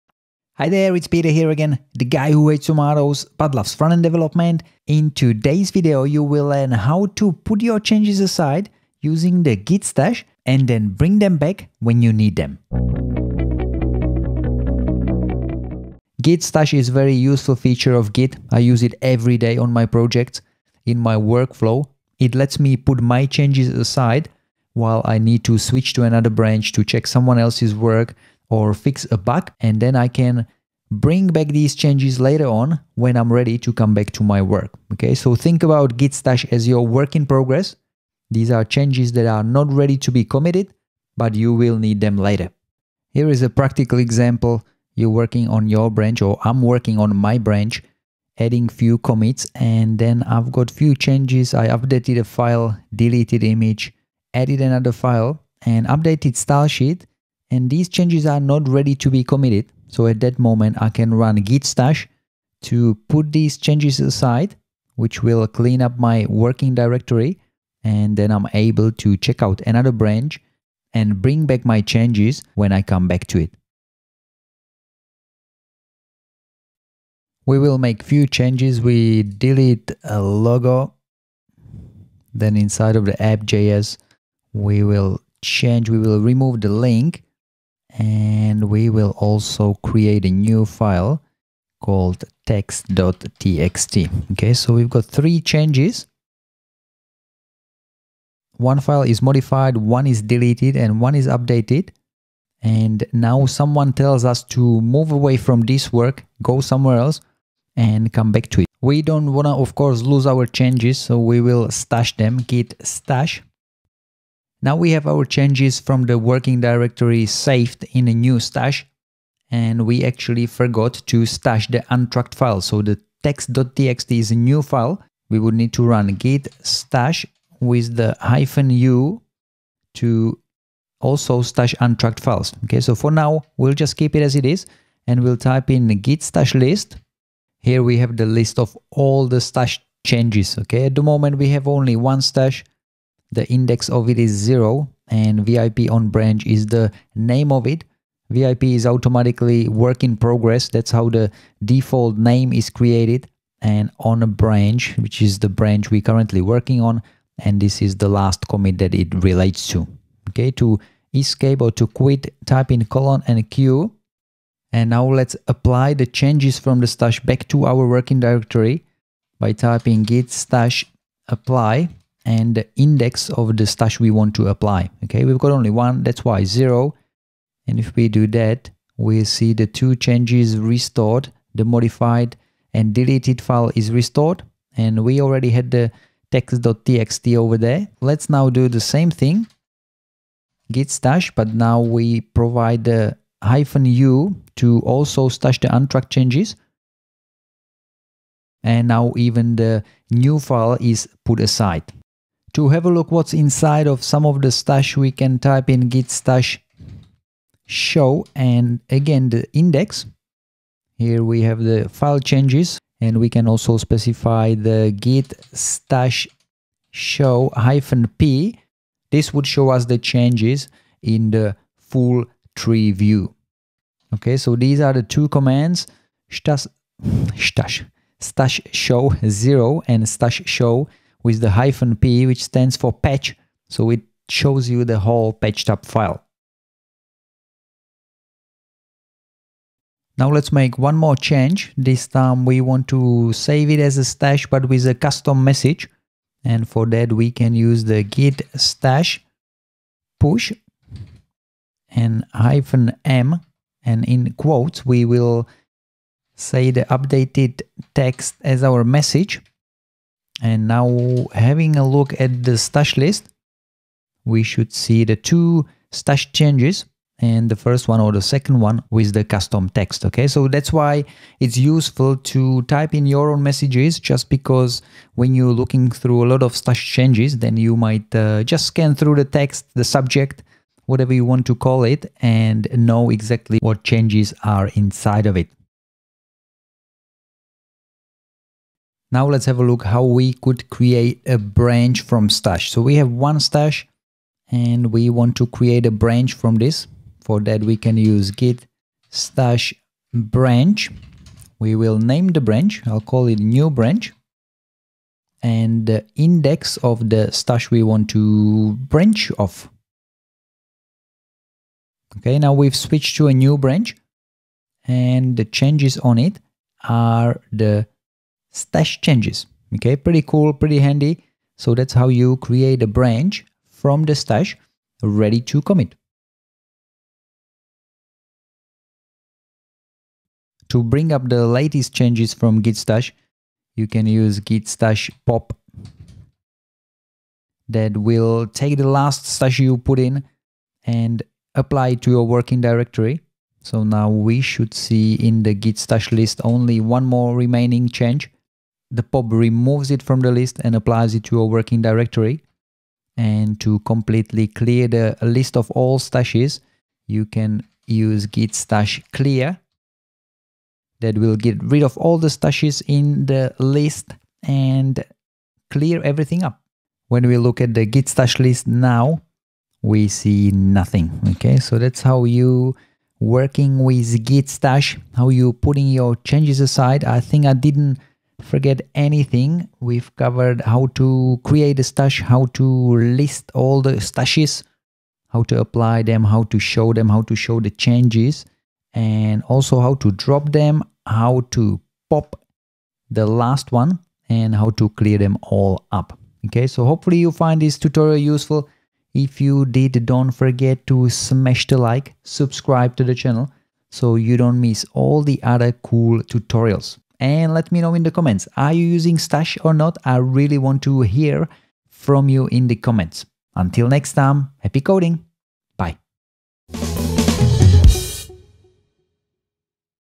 Git stash tutorial. How to save changes for later in git.
Learn how to use git stash to put your changes aside and use them later.

What you will learn in this Git stash tutorial:

- what is git stash
- how to show list of all stashed changes
- how to create, apply and show stash
- how to create a branch from stash

--- Online Courses ---